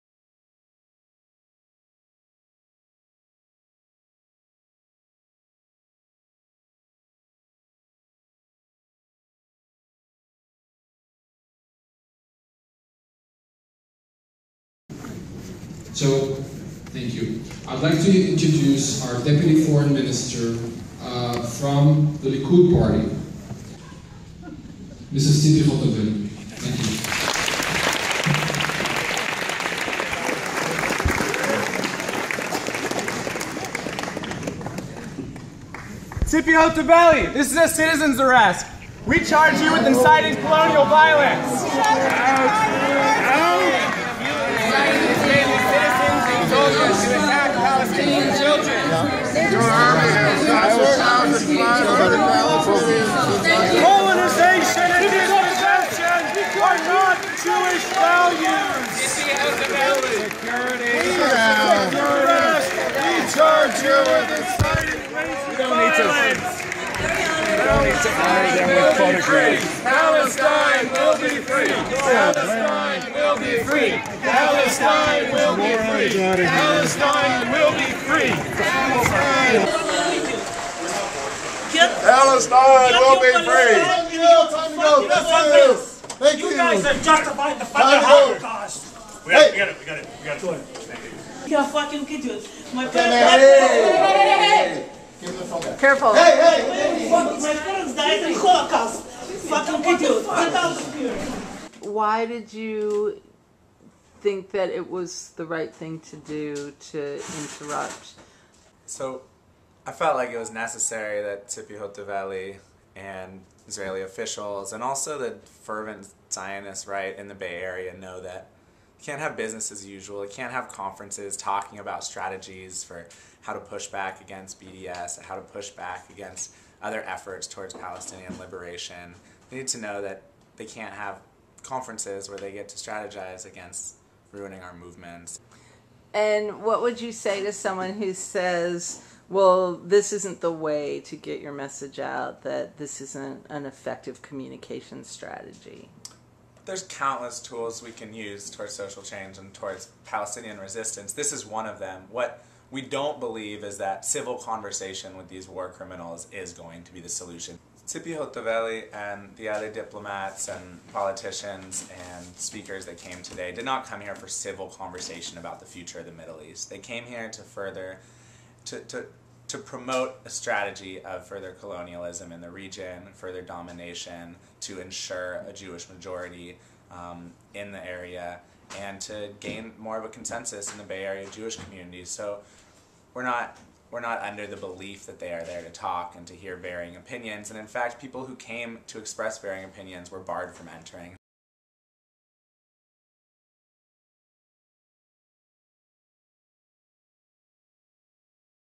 Bay Area Activists
Israeli Deputy Foreign Minister Tzipi Hotovely was an invited speaker at the Zionism 3.0 conference in Palo Alto, California on November 22.  Hotovely is a member of the right-wing Likud party and has called for assassination of Palestinian leaders and annexation by Israel of all the land "between the [Jordan] River and the [Mediterranean] Sea."  As she began to speak, five activists rose from the audience to declare her under citizen's arrest, in keeping with warrants recently issued for Israeli officials in Spain, South Africa and Turkey.